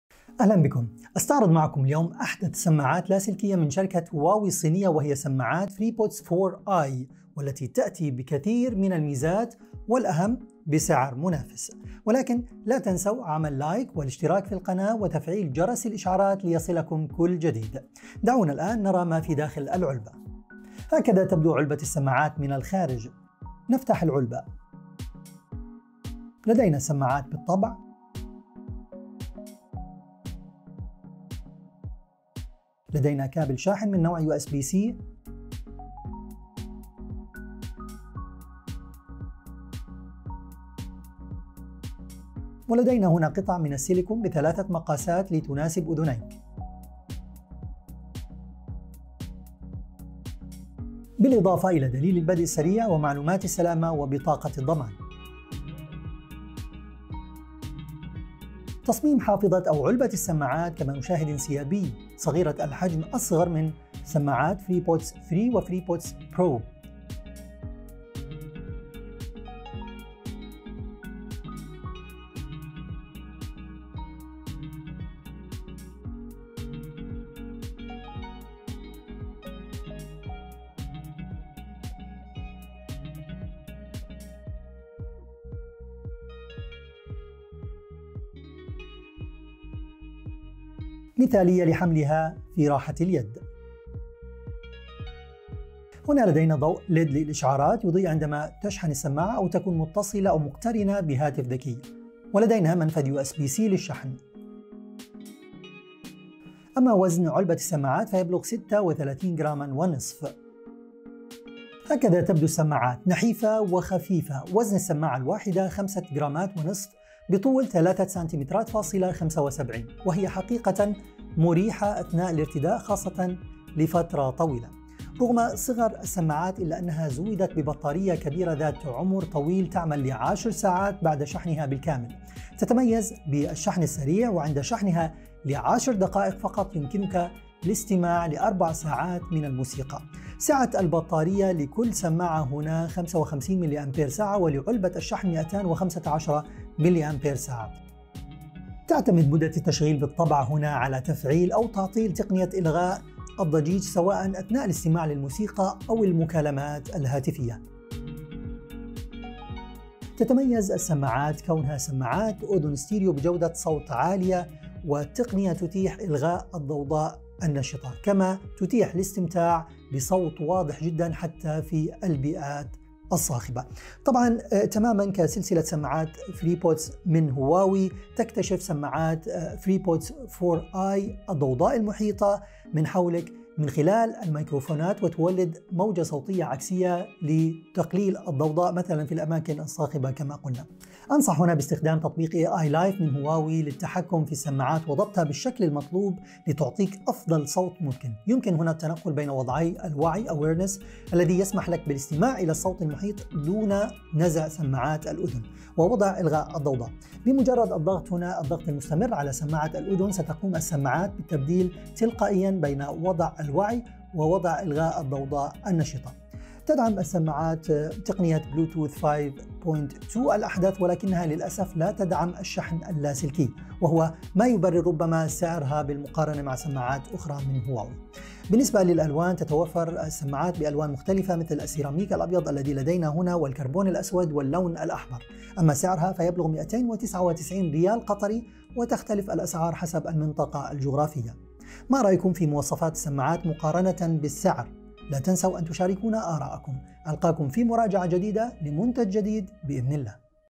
Huawei Freebuds 4i سماعات هواوي فريبودز 4 آي مراجعة
مراجعة لأحدث سماعات لاسلكية من هواوي، ميزات جميلة والأهم بسعر منافس